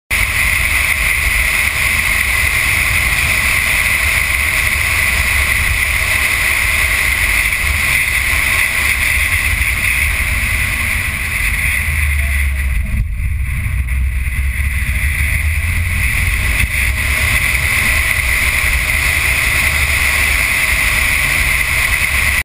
Flatrock Prologue Stage Targa 2012
Coming to the end of Flatrock prologue and what do we find? A pickup truck driving down the road. As we approach, the truck comes to a stop because of the road closures, and then decides to back up in our general direction.